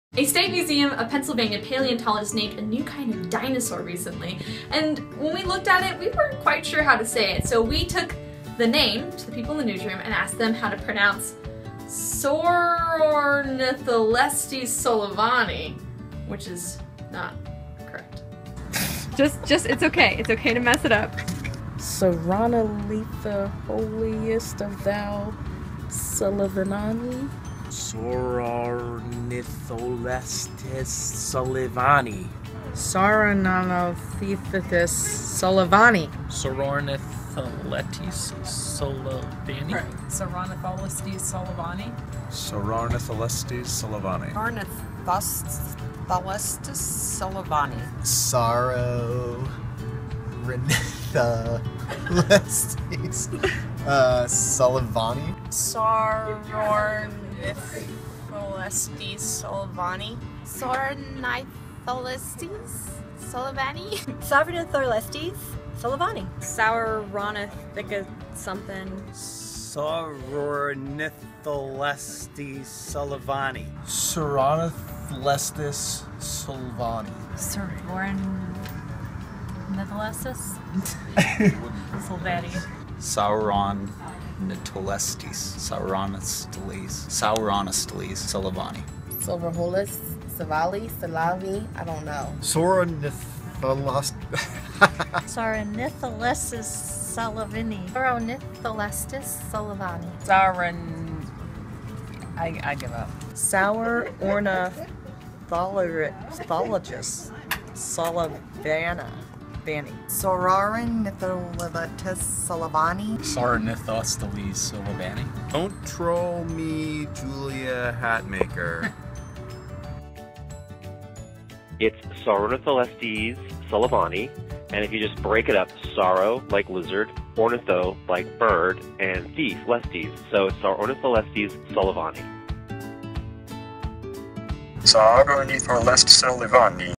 How to pronounce Saurornitholestes sullivani
There&apos;s a new dinosaur, Saurornitholestes sullivani. But how do you pronounce its name? We asked people in the newsroom to give it their best show (and then we had an expert show us how it&apos;s really pronounced).